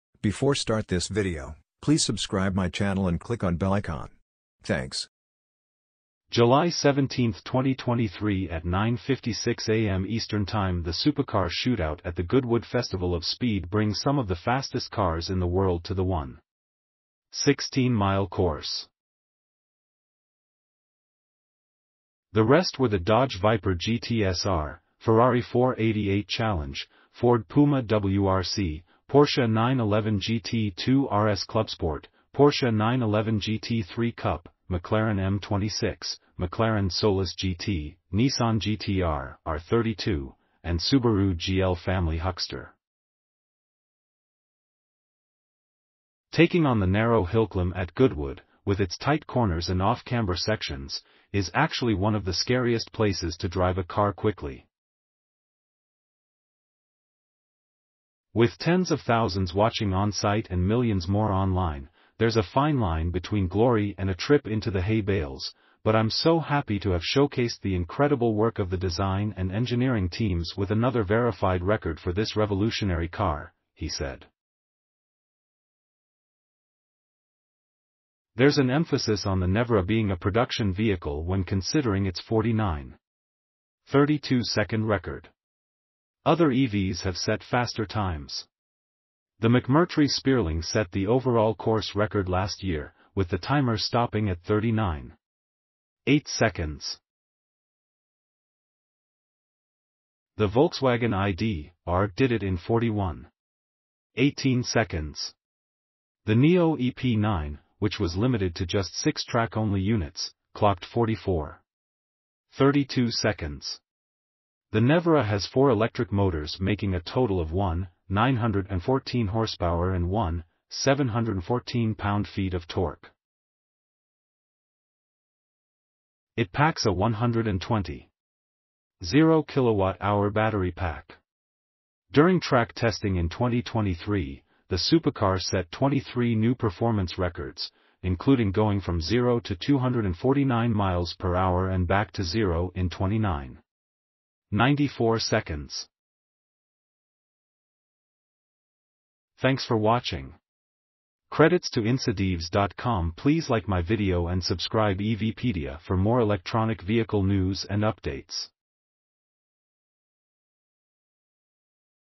See Rimac Nevera Set EV Production Car Record At Goodwood Festival of Speed
Jul 17, 2023 at 9:56am ET The Supercar Shootout at the Goodwood Festival of Speed brings some of the fastest cars in the world to the 1.16-mile course. The McLaren Solus GT blitzed the course in 45.34 seconds, setting the lowest time in 2023. The Rimac Nevera was there too and crossed the line in 49.32 seconds to set a record as the fastest production-spec EV ever on this road. Goodwood splits cars into groups for the Supercar Shootout. The Nevera was the only EV in the class for the fastest vehicles. The rest were the Dodge Viper GTS-R, Ferrari...